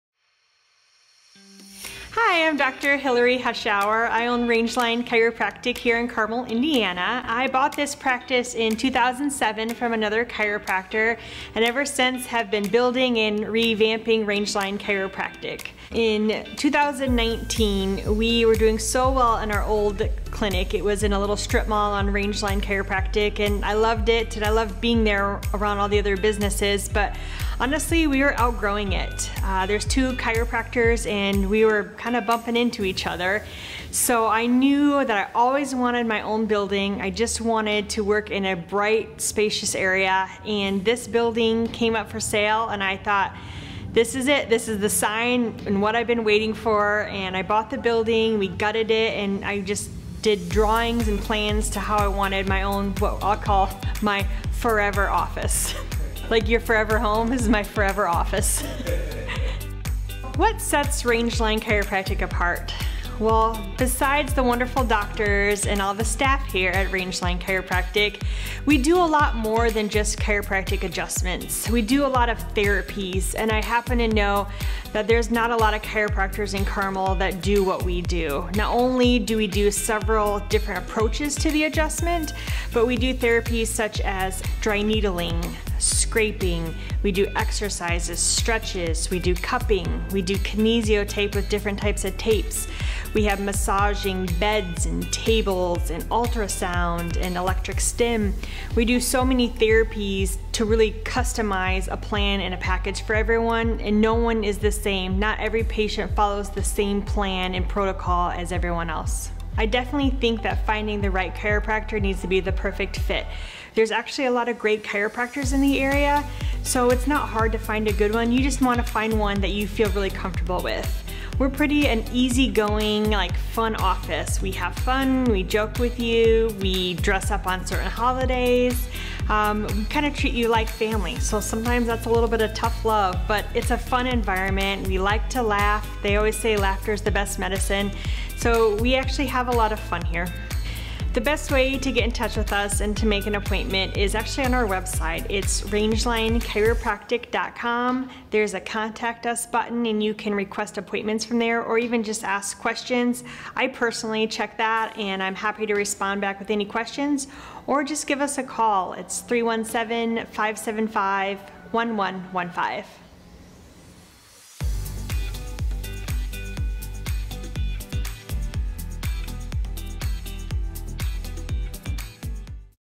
Why Rangeline Chiropractic??
Dr. Hillary Hushower explains how our office is different, because "different" is good, right?